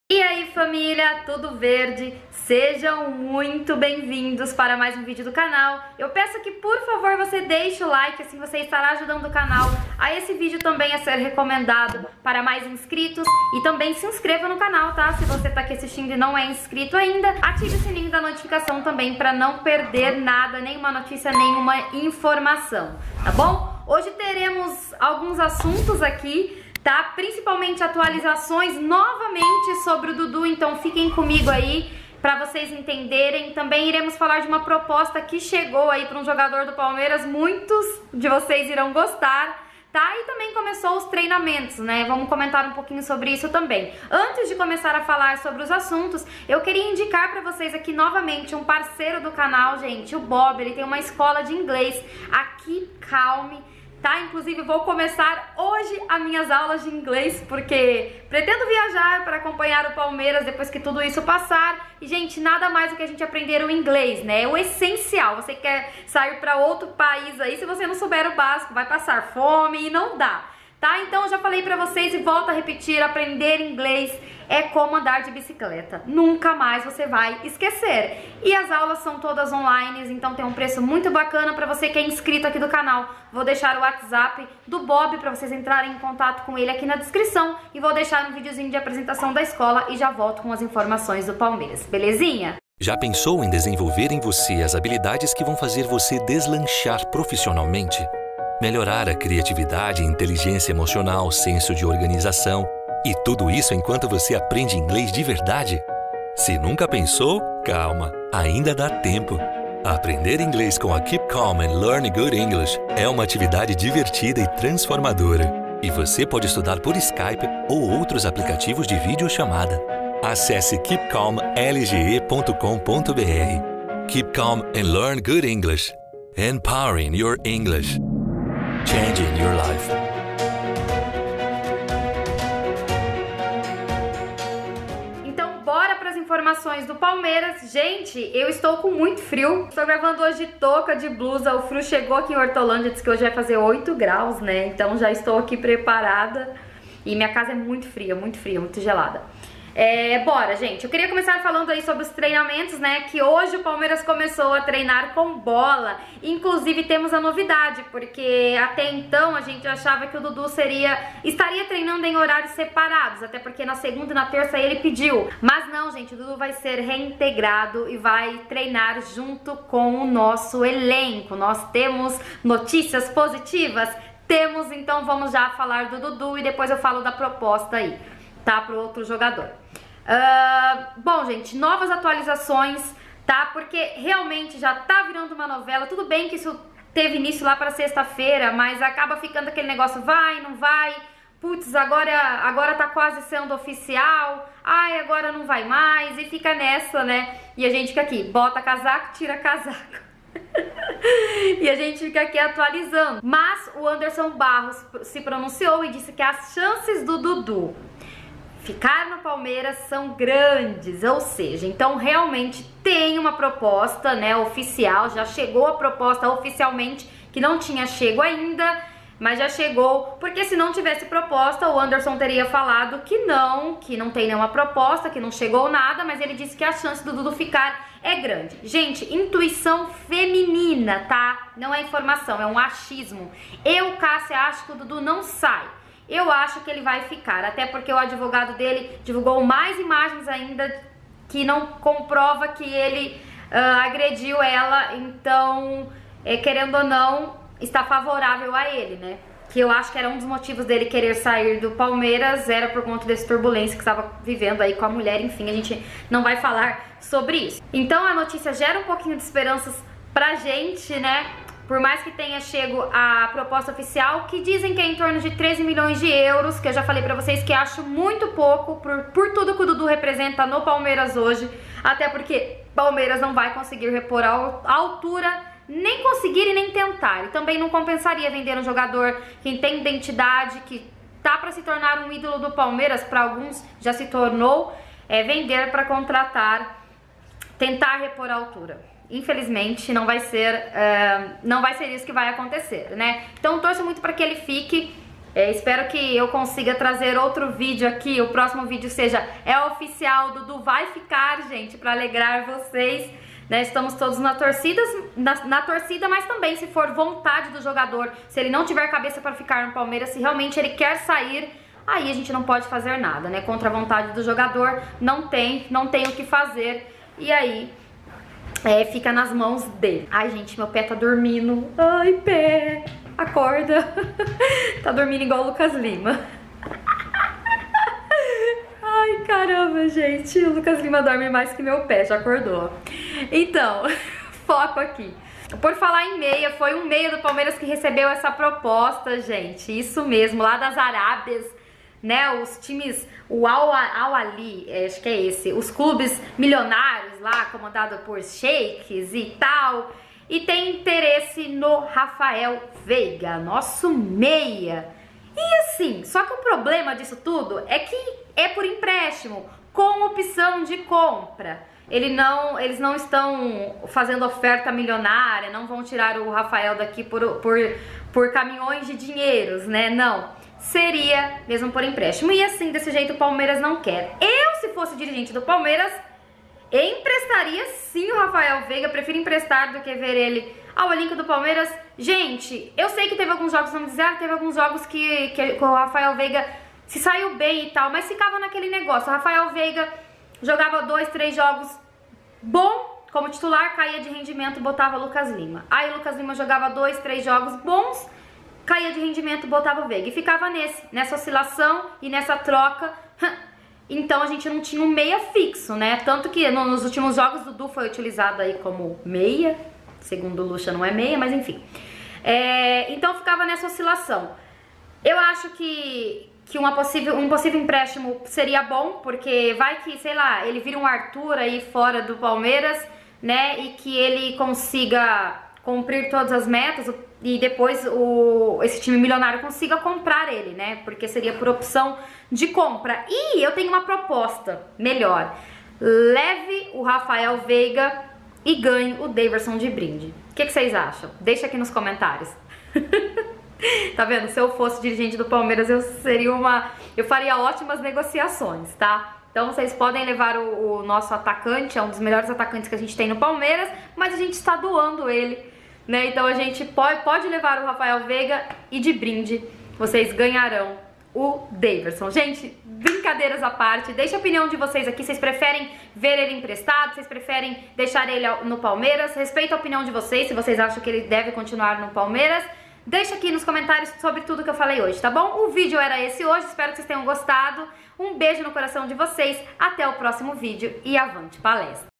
💸CLUBE MILIONÁRIO QUER MEIA DO PALMEIRAS | OFICIAL: ANDERSON BARROS FALA DE DUDU | VOLTA DOS TREINOS
WHATS DO BOB: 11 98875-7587

Conta Itaú :
Kassia Alves Poggi
AG: 2078
CONTA: 34889-9